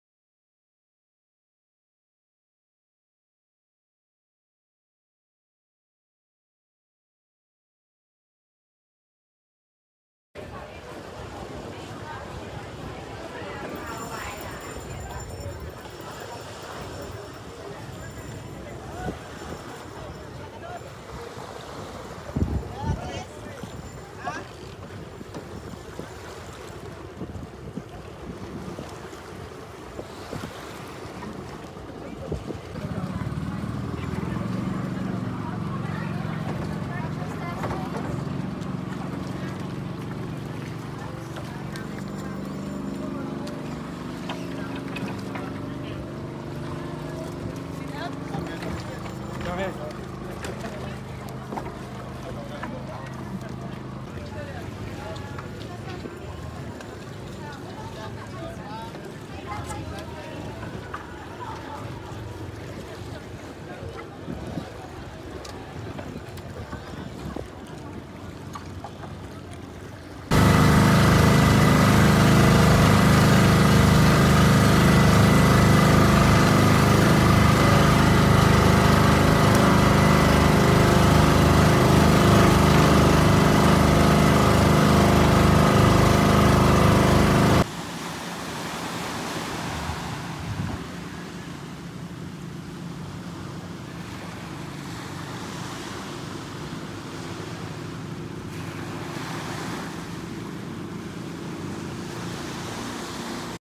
Sabang Port to Puerto Princesa Underground River
Sabang Boat Terminal transports visitors via motorized banca to the Underground River.